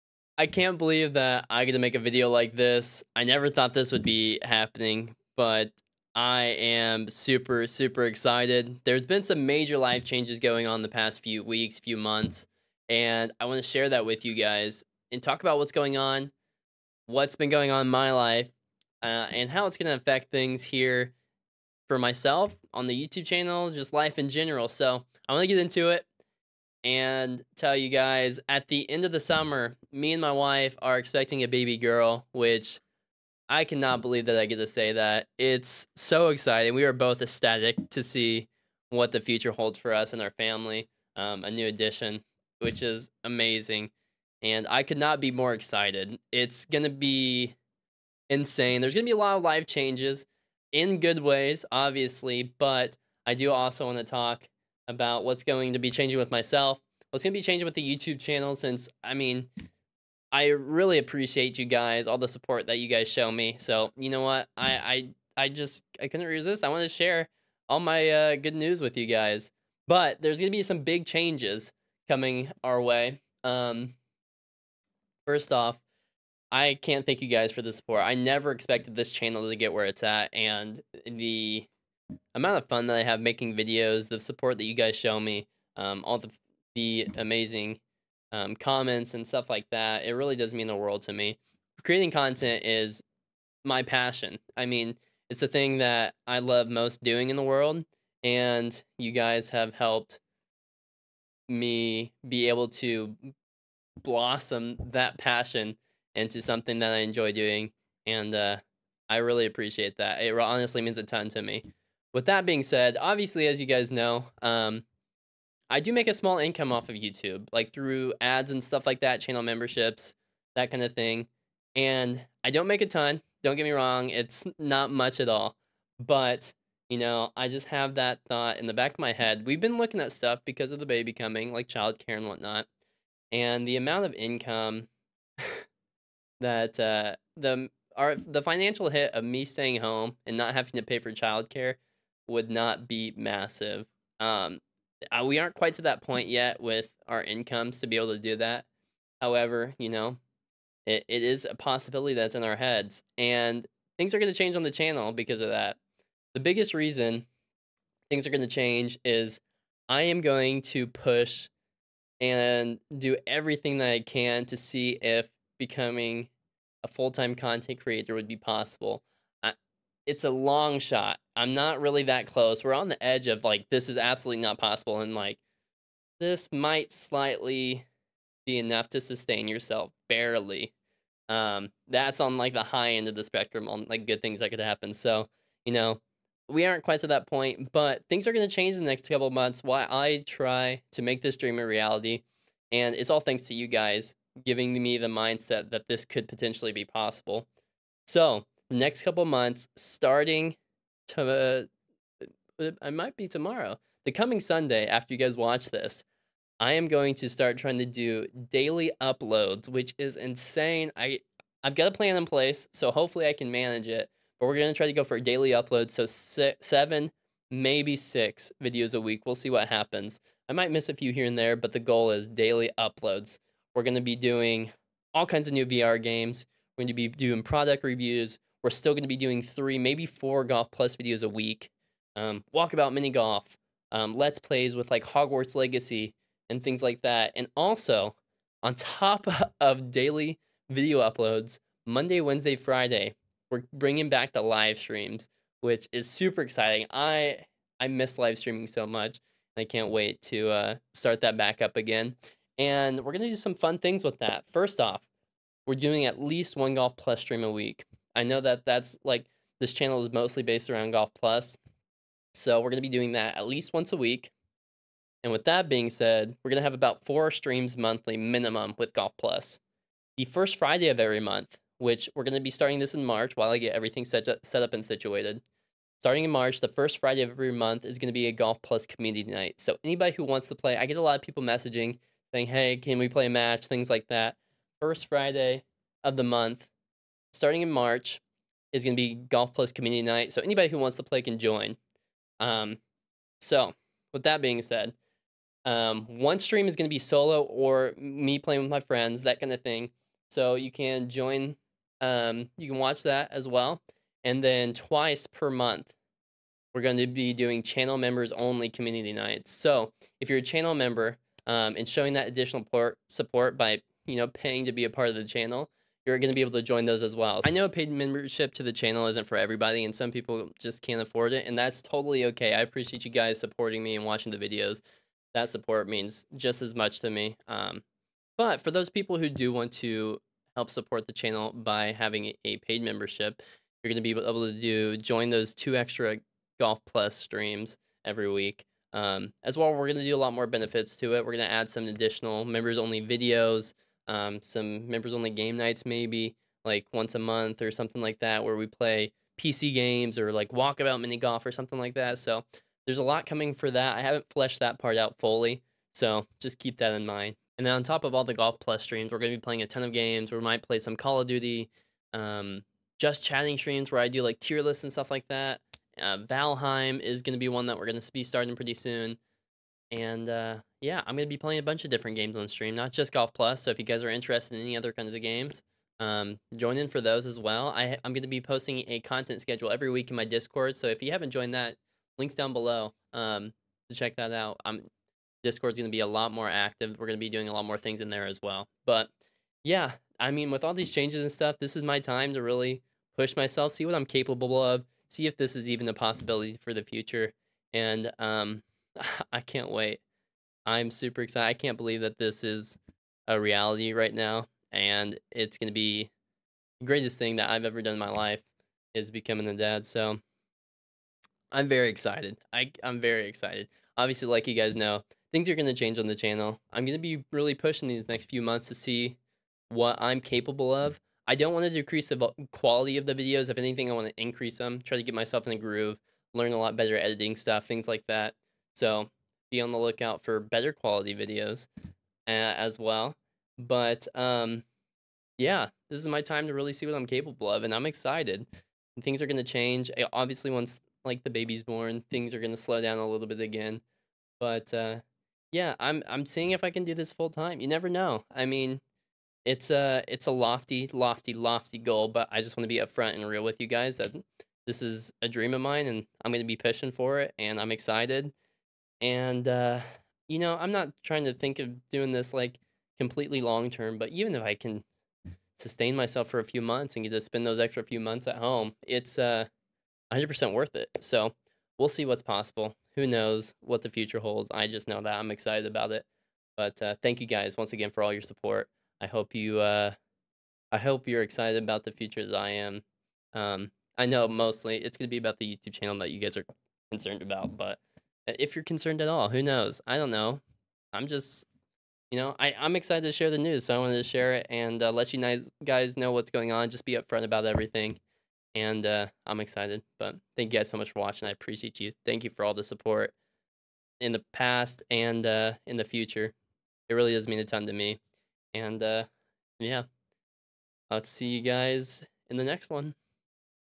2024 Channel and Life Update
Head strap I use
Extra battery
Face pad for quest 2
Quest 2 controller grips

Timestamps:
0:00 Intro
0:25 Announcement
1:50 Full Time?
3:15 Video Uploads
3:58 Live streams and Golf+ community nights
6:28 Thank you